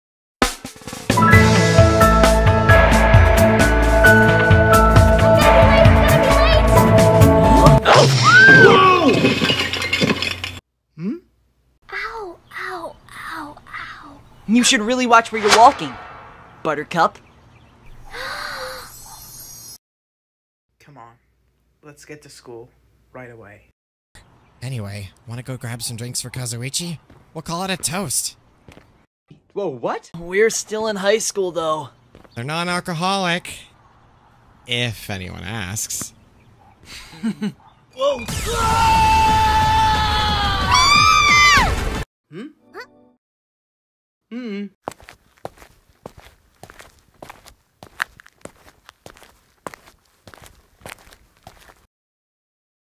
LUKE CHEERS UP SAKURA AFTER THE MISTAKE
IT WAS FRIDAY JUNE 10TH 2022 THE DAY WHEN ￼JURASSIC WORLD 3 WAS OUT SO I CHEER UP SAKURA (MY GIRLFRIEND) AFTER SHE ACCIDENTALLY HIT NATHAN OVERWALL